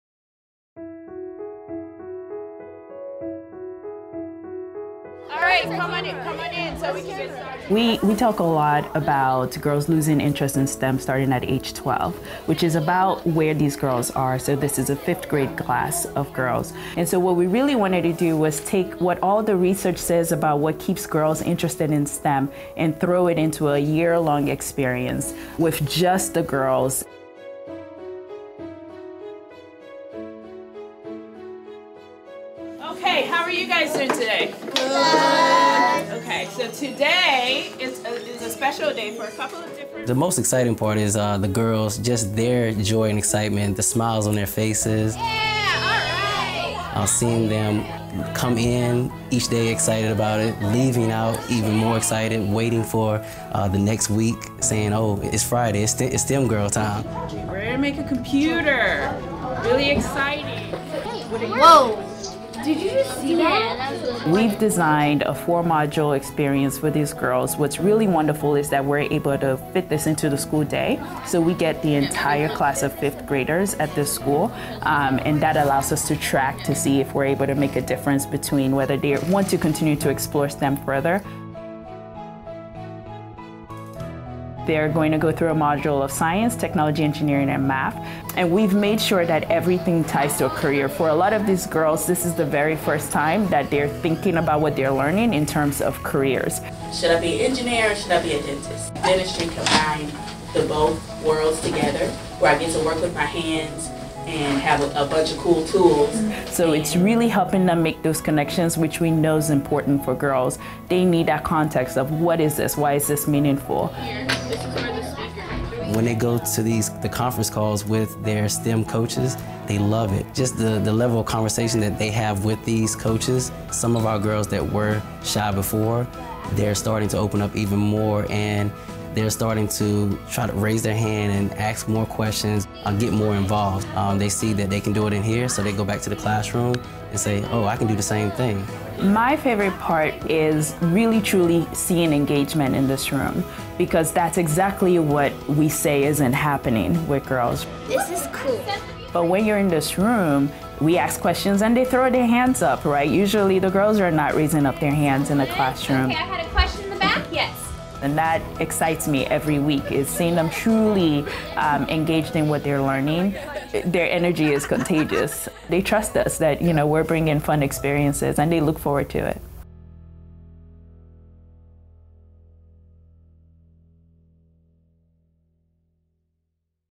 Smart Girls HQ
The factors promoting the underrepresentation of girls and women in the STEM talent pipeline often begins early in their education. Despite showing the same levels of ability and aptitude in STEM coursework, girls begin to show less interest in math or science careers when compared to their male peers by age 12.

Skookum is a proud sponsor of Smart Girls HQ, an organization that focuses on STEM education specifically tailored towards young girls. Take a look at how this program is reaching young students and teaching them how they can thrive in science, technology, engineering, and math.